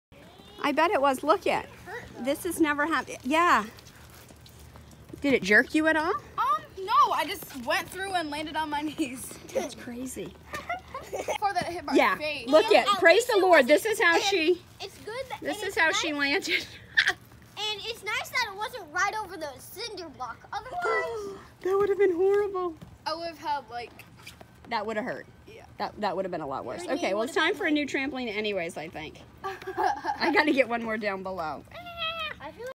Trampoline Broke 2019
Of all the years and all the trampolines this is a first!!! Glad everyone was okay.